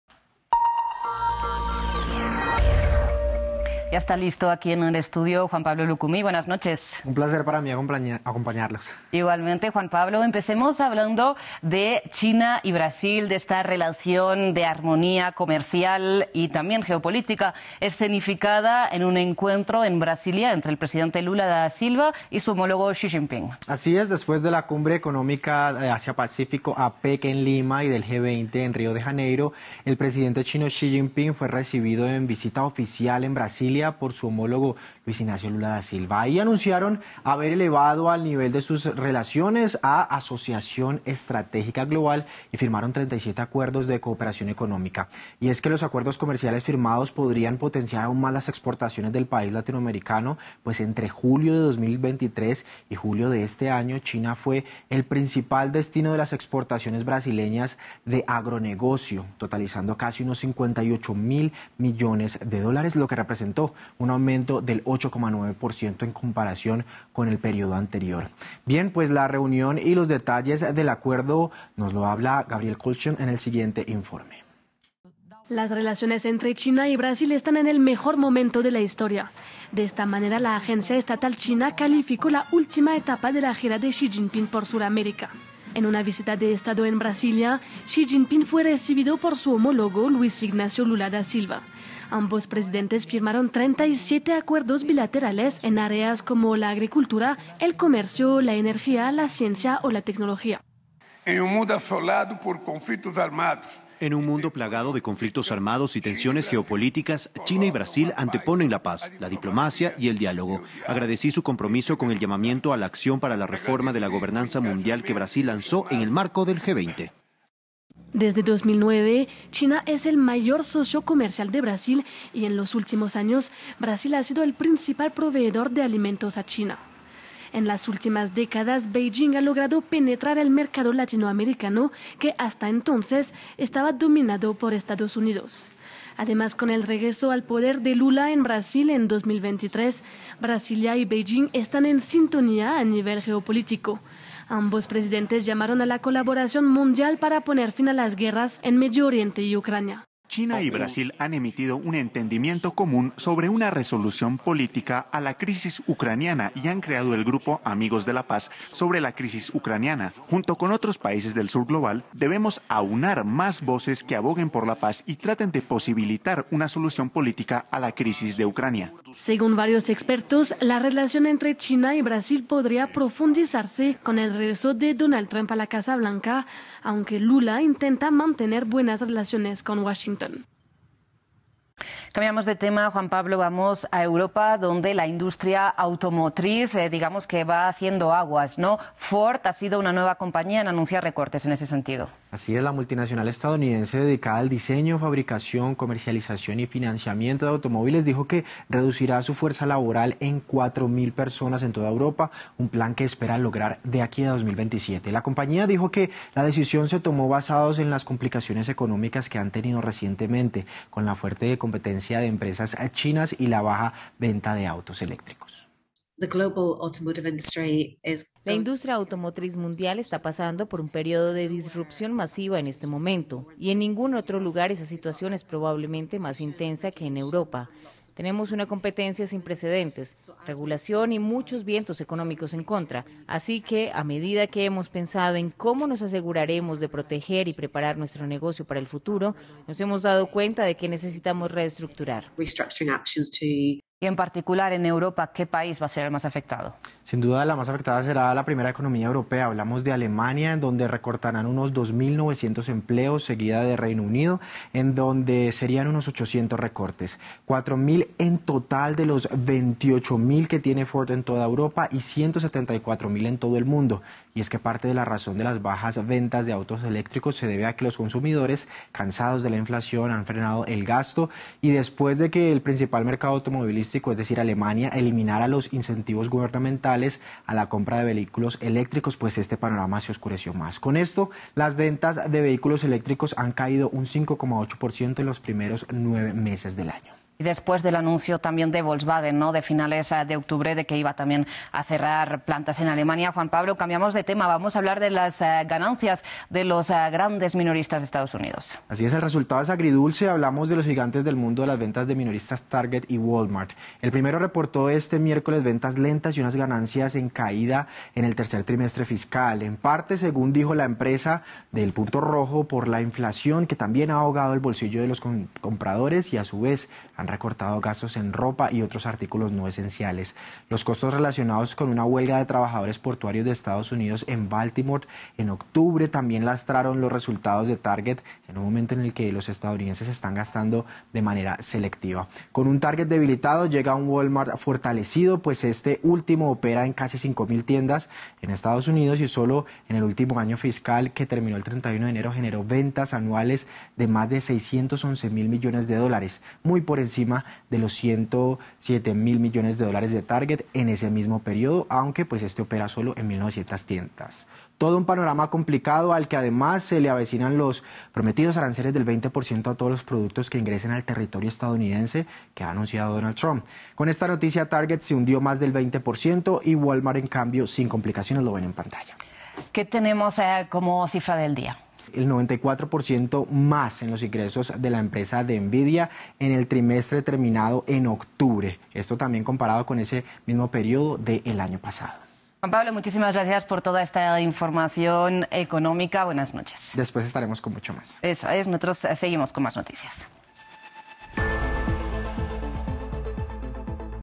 Brasil y China elevan su alianza estratégica a nuevas alturas globales
Los presidentes de ambos países anunciaron haber elevado sus relaciones al nivel de ‘Asociación Estratégica Global’ en una reunión oficial en Brasilia luego la cumbre que reúne a las economías más poderosas del mundo, G20, celebrada en la capital brasileña. En el encuentro los mandatarios firmaron 37 acuerdos que van desde comercio y turismo hasta agricultura, industria, ciencia y tecnología, salud, energía, cultura y educación.